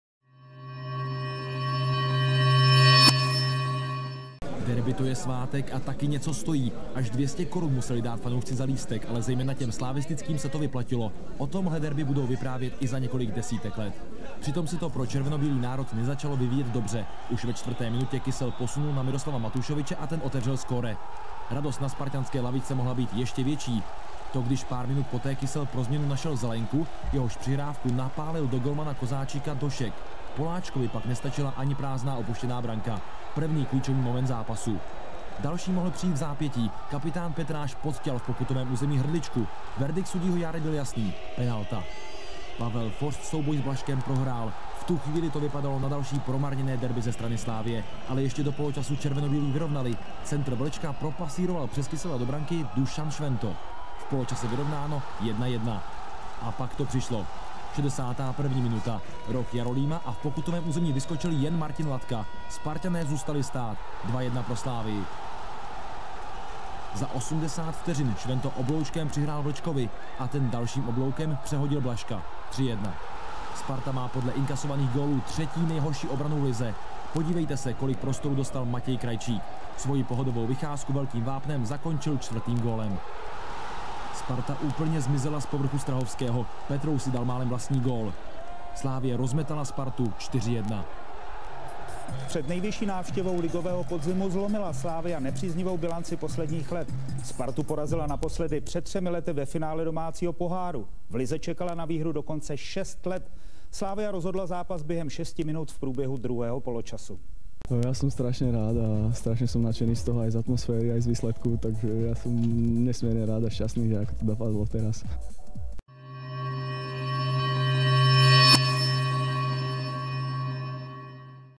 Slavia - Sparta 4:1 - Dušan Švento gól a rozhovor
Podívejte se na reportáž ze zápasu Slavie se Spartou a rozhovor se Šventem!
Branky: 31. Švento, 62. Latka, 63. Vlček, 67. Krajčík - 5. Matušovič

Sestava Slavie: Kozáčik - Krajčík, Latka, Suchý, Zbončák - Victorino (36. Švec), L. Jarolím (79. Kratochvíl), Hrdlička, Švento - Vlček, Fořt (84. Pešír). Trenér: K. Jarolím